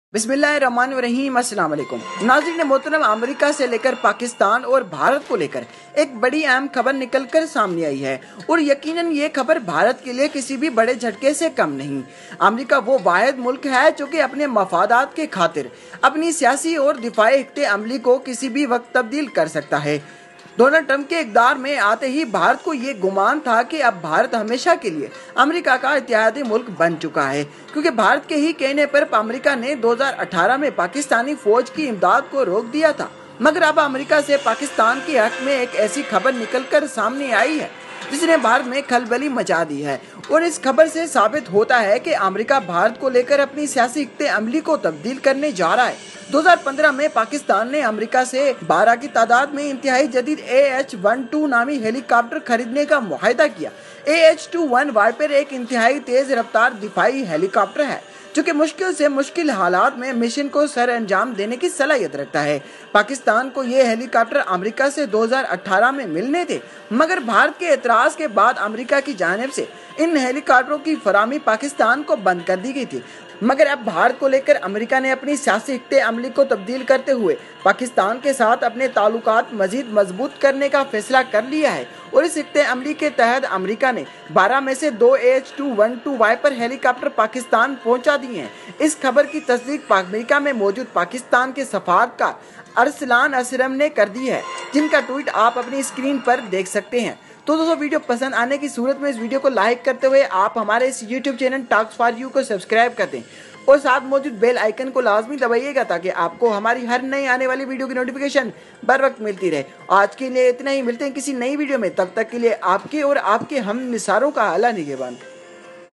Power Of Pakistani AH-1Z Helicopters and Pakistan F-16 Fighter Aircraft
Power Of Pakistani AH-1Z Viper Helicopters and Pakistan F-16 Fighter Aircraft

Pakistan Received Two Ah-1z viper Helicopter